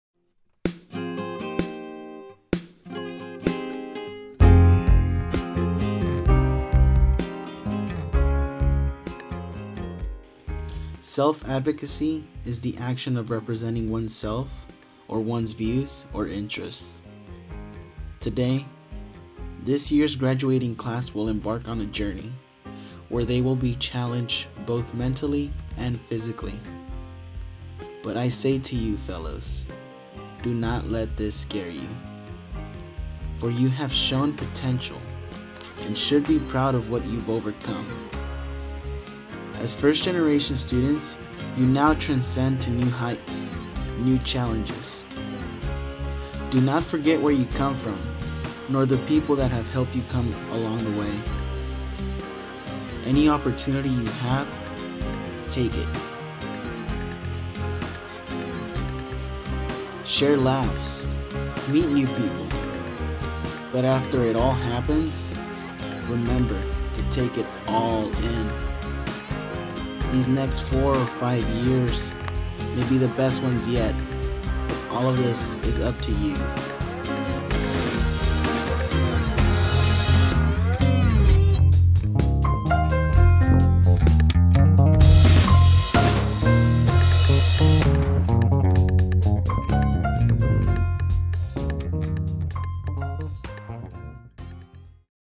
Self-Advocacy - It's Your Moment, Take It!
As part of the EMERGE program, I have learned to appreciate the opportunities that I have earned after high school and wanted to relay the message to the EMERGE Fellows Class of 2018.
EMERGE empowers & prepares high-performing students from underserved communities to attend and graduate from selective colleges & universities across the nation.